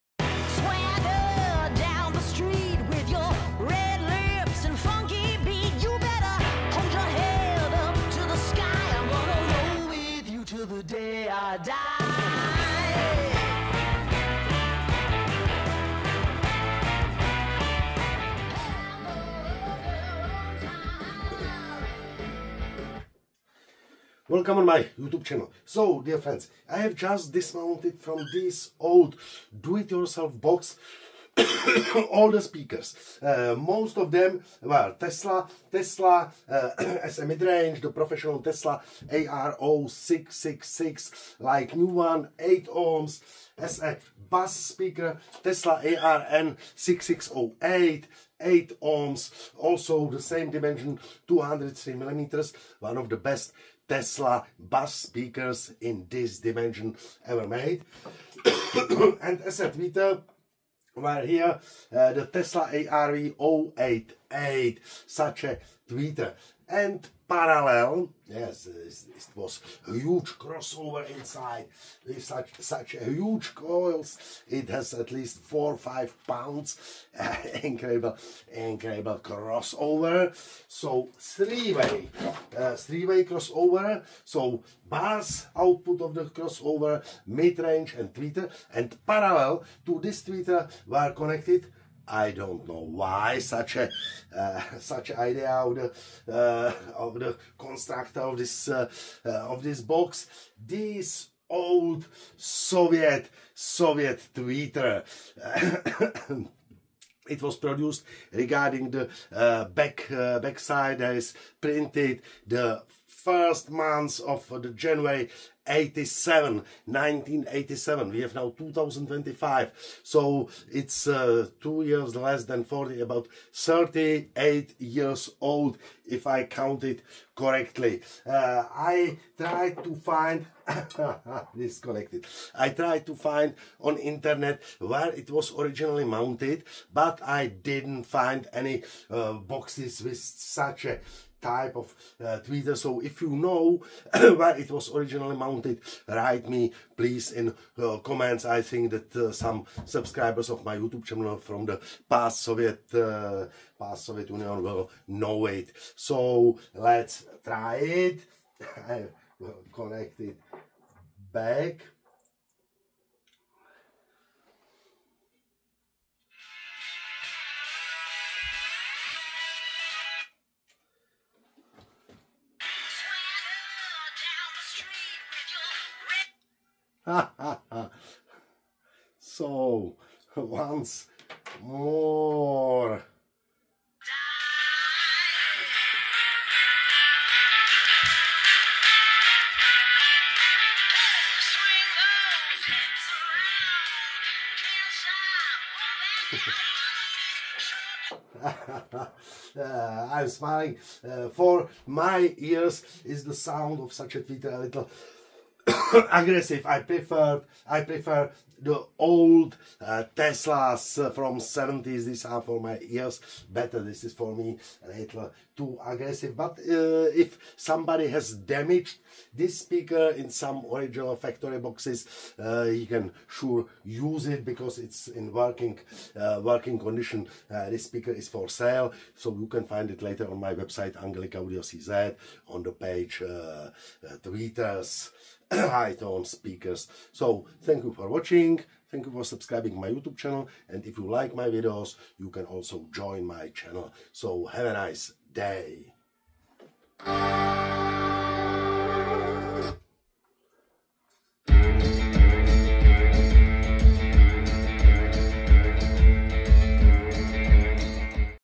80s Vintage USSR Soviet TWEETER 4 ГДВ-1-8 (3 ГД 47) Speaker - 15АС-214, 15АС-303А, 15АС-8, 25АС-208
80s Vintage USSR - Soviet TWEETER 4 ГДВ-1-8 (3 ГД 47) - 4GDV-1-8 (3GD47) Speaker Sound Test @Angelicaaudio 179388 @youtubecreators
Высокочастотный динамик. Купол сделан из металлизированного лавсана. Каркас звуковой катушки выполнен из бумаги. Купол  рупорный.
Те. характеристики:
Диапазон частот: 2000 – 20000 Гц
Неравномерность частотной характеристики: 14 дБ
Уровень характеристической чувствительности: 91 дБ
Номинальное сопротивление: 8 Ом
Предельная паспортная мощность: 4 Вт
Максимальная мощность: 6 Вт
Частота основного резонанса: 1500 Гц
Размеры: 65х65х44 мм.
Больше информации по ним нет. Выпускался он очень небольшими тиражами. Производились, по непроверенной информации, в г. Великий Березный. Ставились в 15АС-214, 15АС-303А (“Ферроприбор”, Ленинград), 15АС-8, 25АС-208.